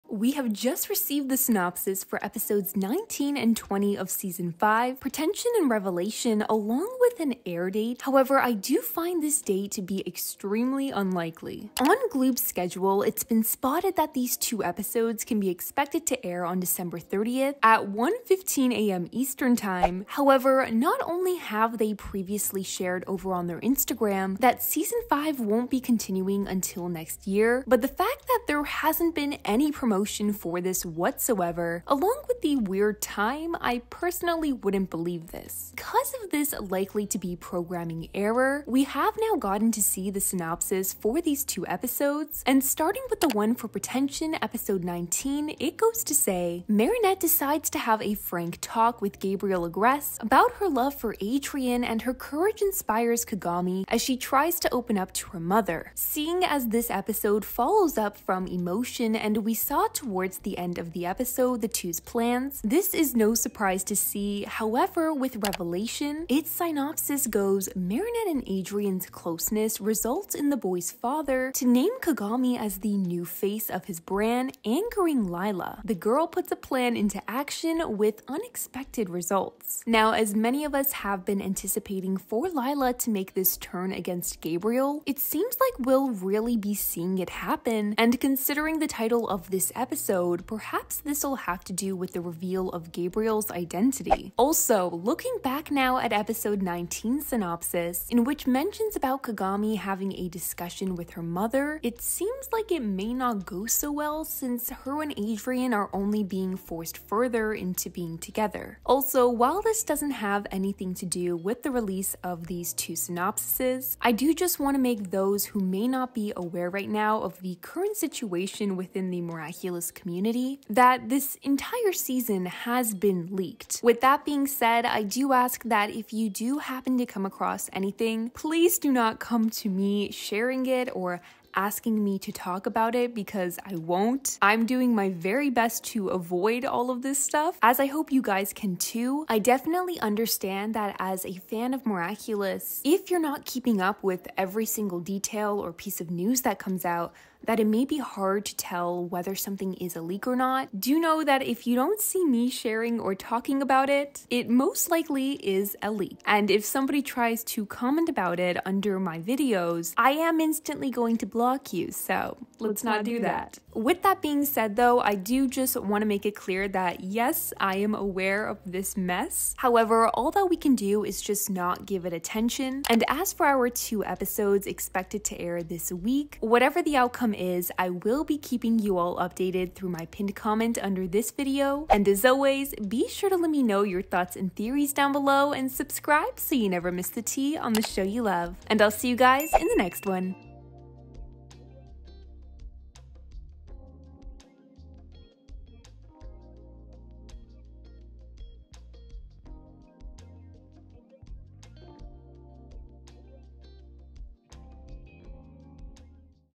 Miraculous Ladybug Season 5 Pretension And Revelation Synopsis’s + Possible Release Date
Let's talk about the reveal of episodes Pretension and Revelations synopsis’s and their possible release date! 👀

Please do not copy or re-upload my videos or thumbnails on any platform.

THANK YOU  X  168K! 🥰 ✨